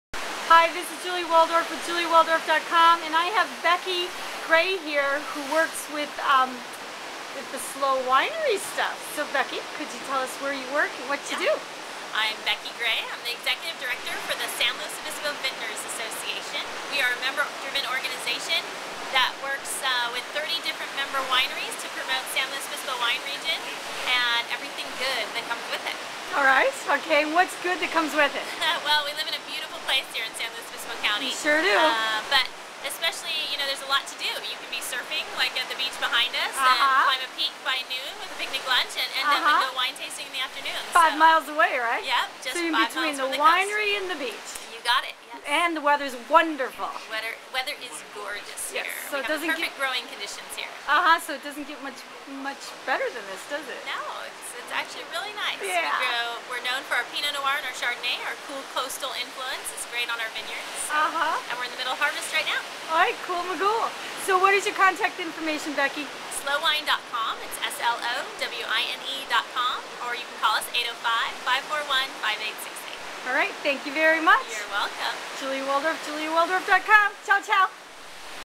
Slo Vineyards with Becky Grey
Promoting Wine grape growing in SLO County with SLOWINECOM
for more fun, news, what to do in Central Coast of California, view properties pismo beach, shell beach, avila beach